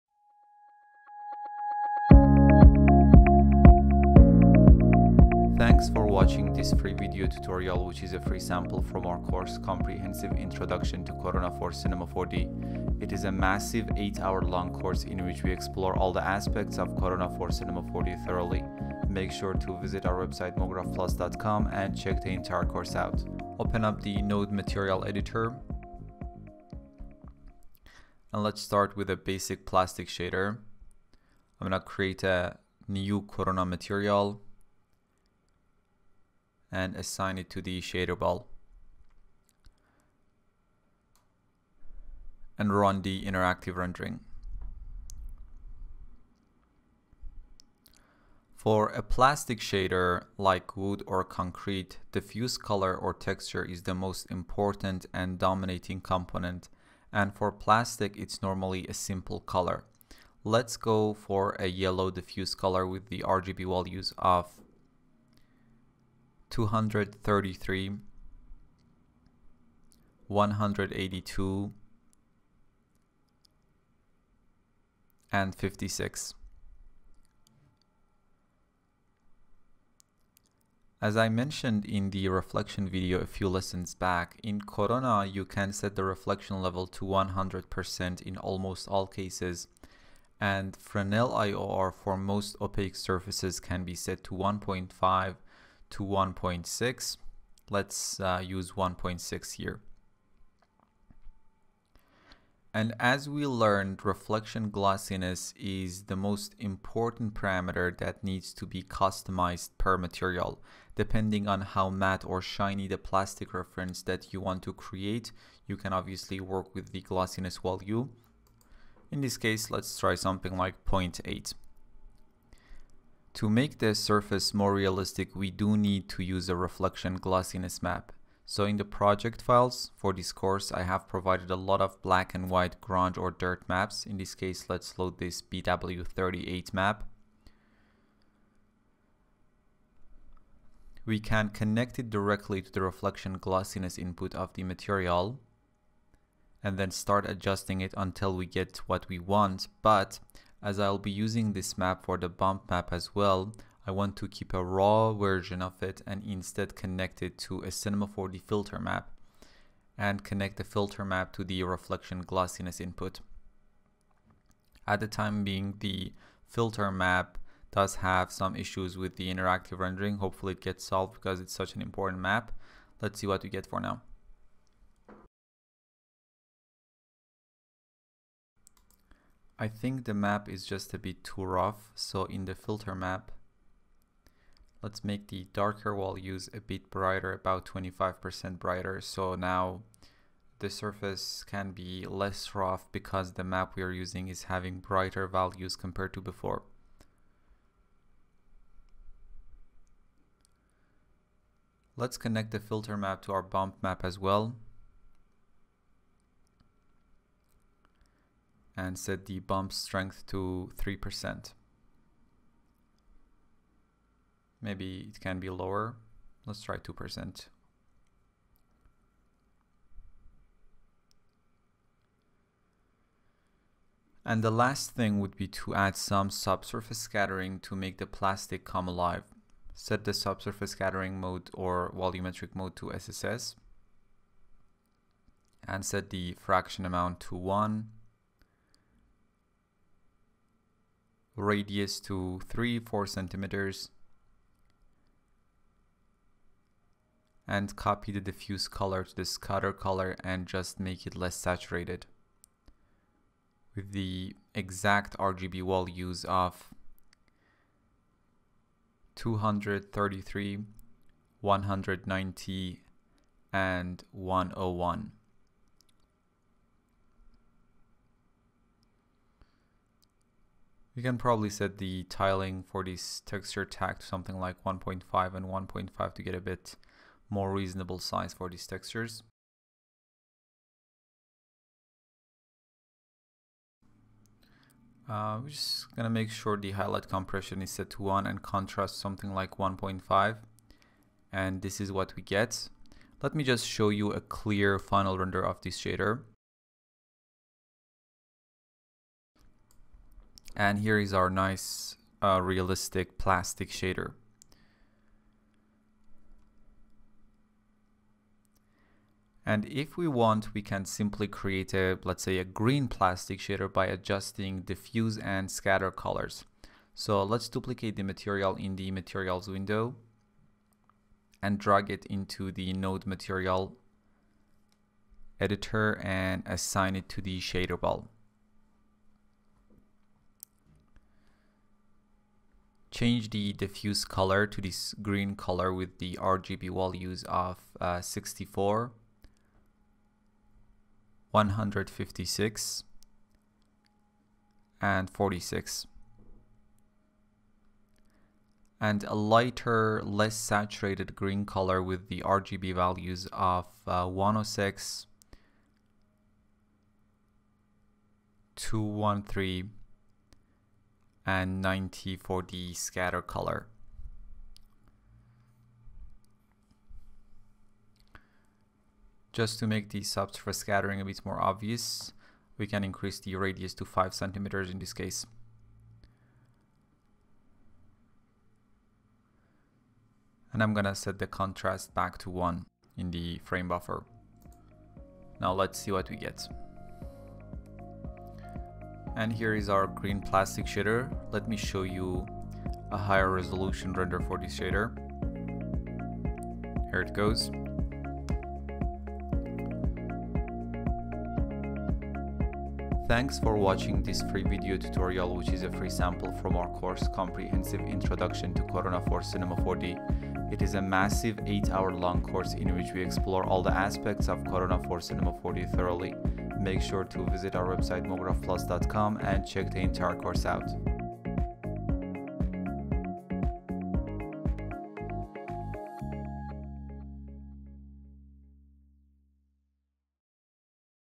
Corona for Cinema 4d | How to make Realistic Plastic Materials
In this video tutorial, we learn how to create realistic plastic material using Corona for Cinema 4d.